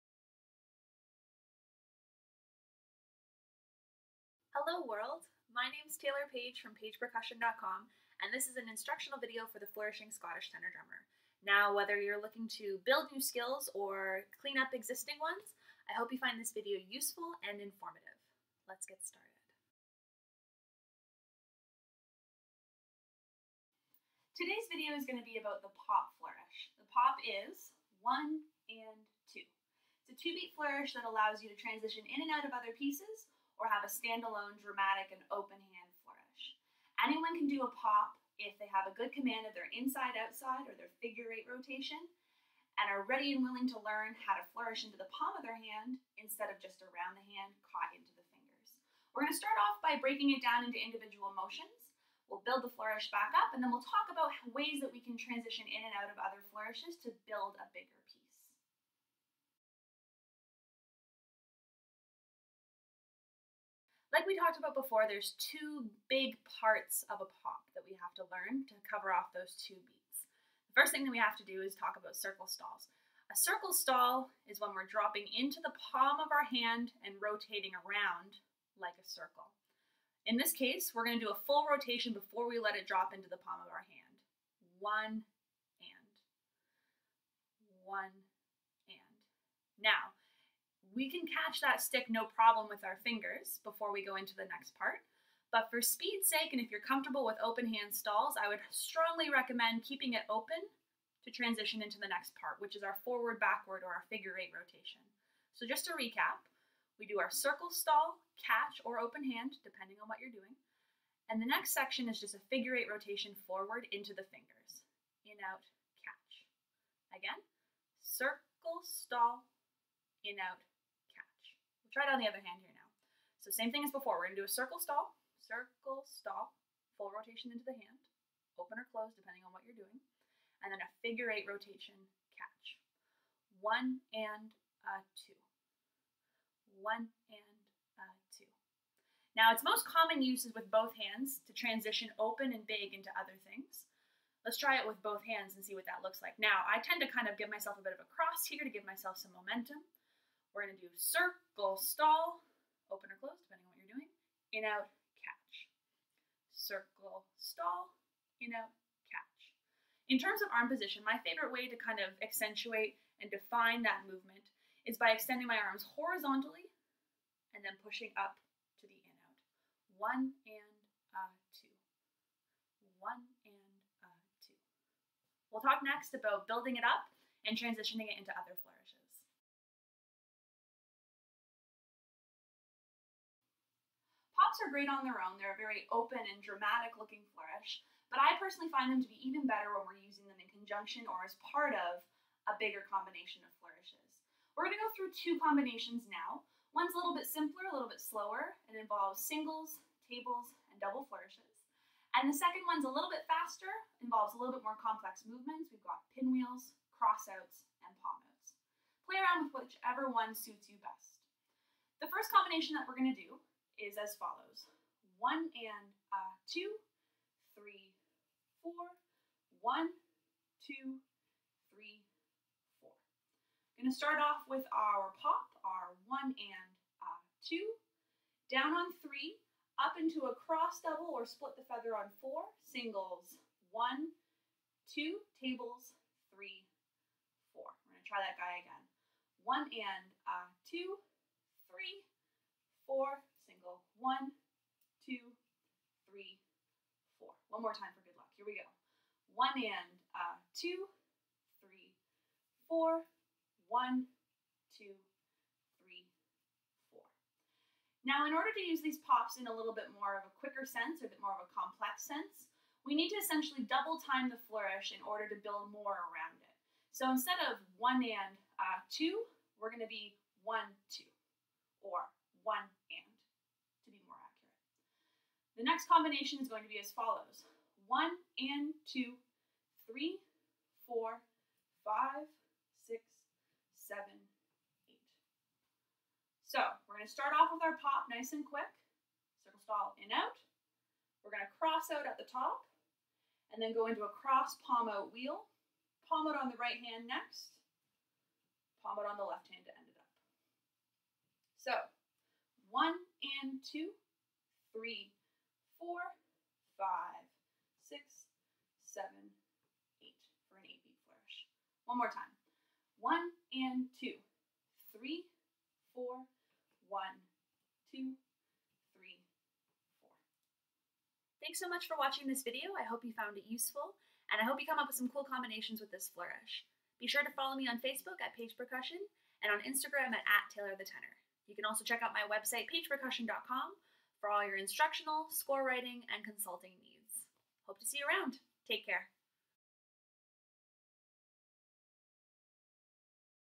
The Pop - Flourishing Instructional Video
Learn a new flourish! Taylor Page from pagepercussion.com teaches you the Pop flourish, breaking it down into small steps and building it up to use in different combinations.

For more information about Taylor, visit pagepercussion.com
Follow Taylor on Facebook at Page Percussion, and on Instagram at @taylorthetenor.